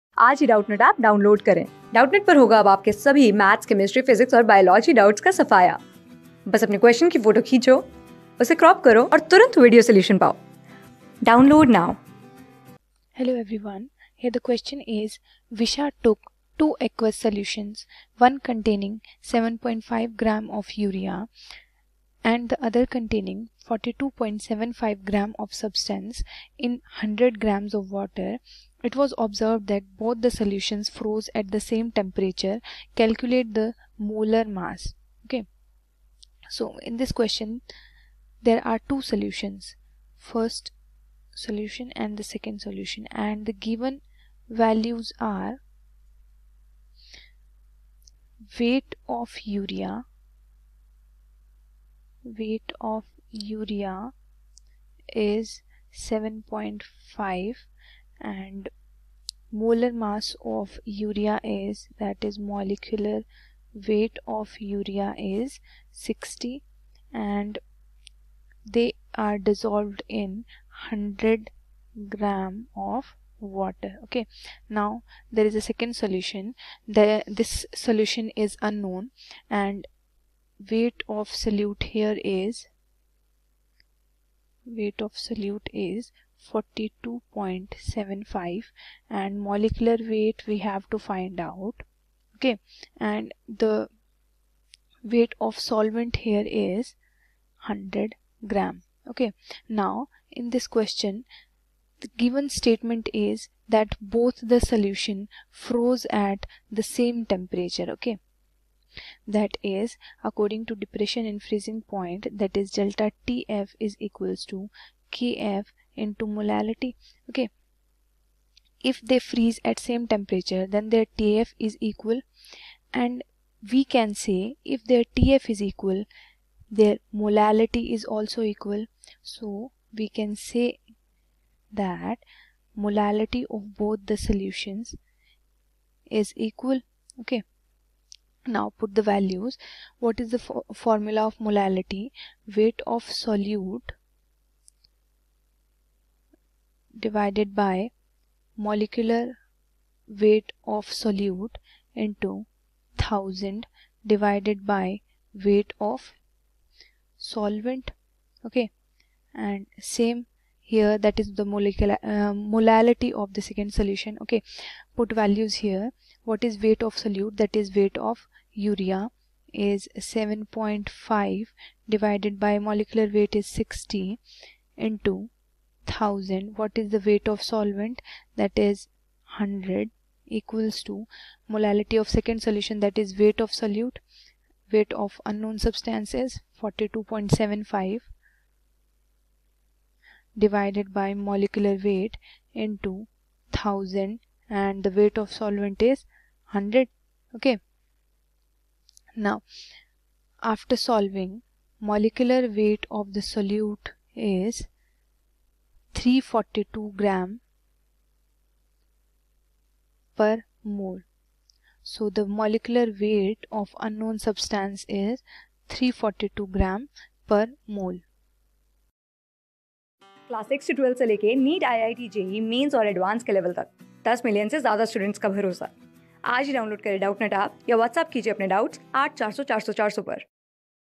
Visha took two aqueous solutions, one containing 7.5 g of urea (Molar mass = 60 g/mol) and the o...
Visha took two aqueous solutions, one containing 7.5 g of urea (Molar mass = 60 g/mol) and the other containing 42.75 g of substance Z in 100 g of water, respectively. It was observed that both the solutions froze at the same temperature. Calculate the molar mass of Z.
Class: 12
Subject: Chemistry
Chapter: XII Boards
Board:CBSE

👉 Have Any Query? Ask Us.